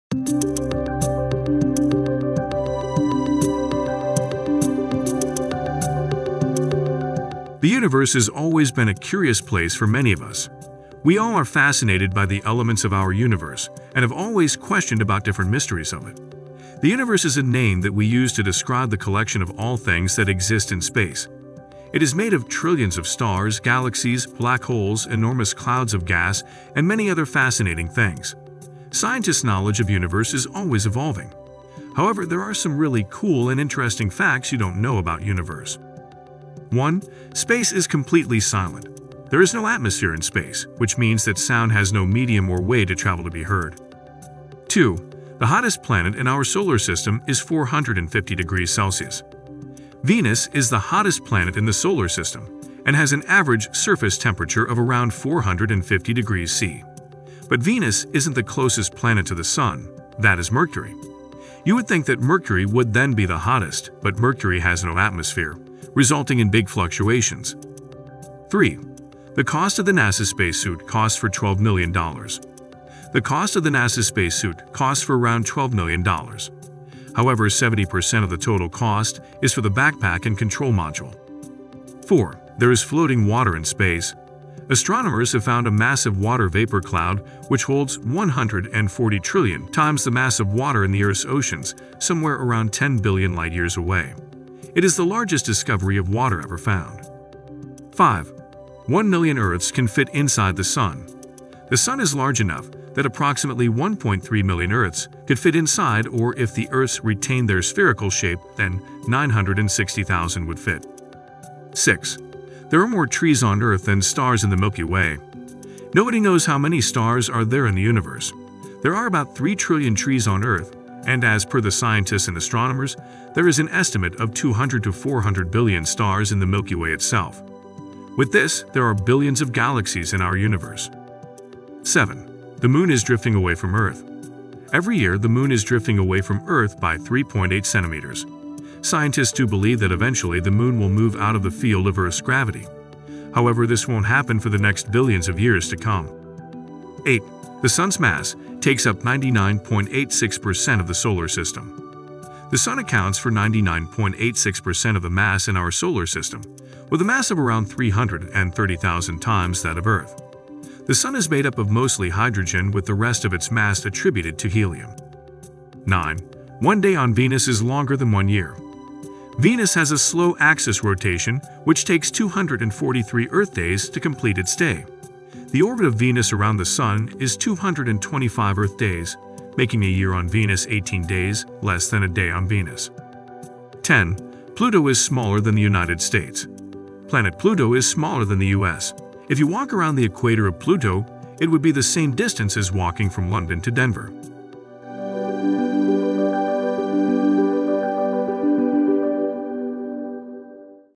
10 Interesting Facts About Our Universe
Embark on a cosmic journey through the wonders of our universe, a vast expanse filled with trillions of stars, galaxies, black holes, and mysterious gas clouds. While scientists continue to uncover new mysteries every day, some astonishing facts about space remain unknown to many.

In this video, we reveal 10 incredible and little-known truths about the cosmos that will expand your understanding of the world beyond our planet. Prepare to be amazed by the secrets hiding in plain sight across the universe!